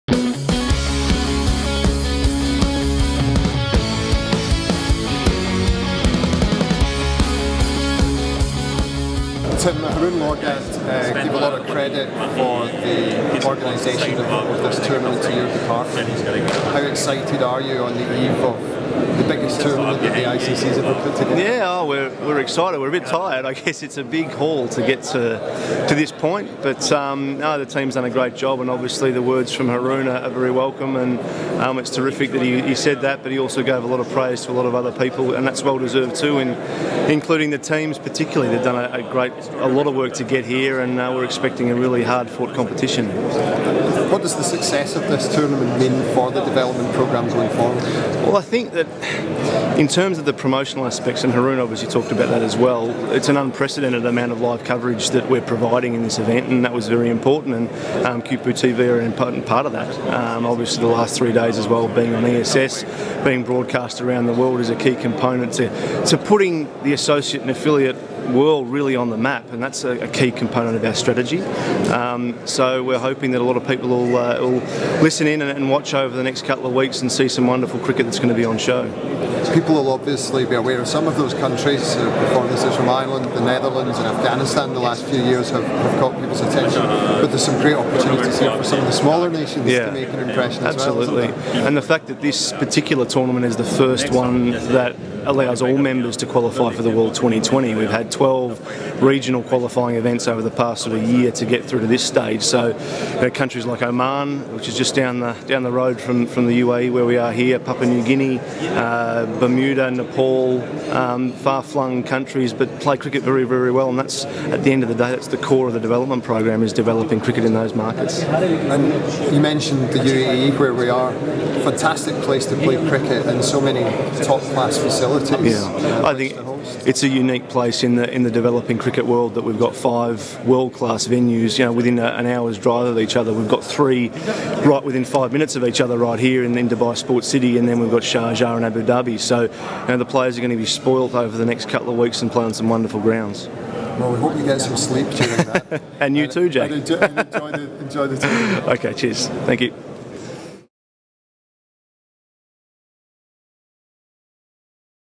Tim Anderson - ICC World Twenty20 Qualifer
ICC Global Development Manager, Tim Anderson, is the driving force behind the T20 Qualifiers and the coverage provided by QuipuTV and ESS.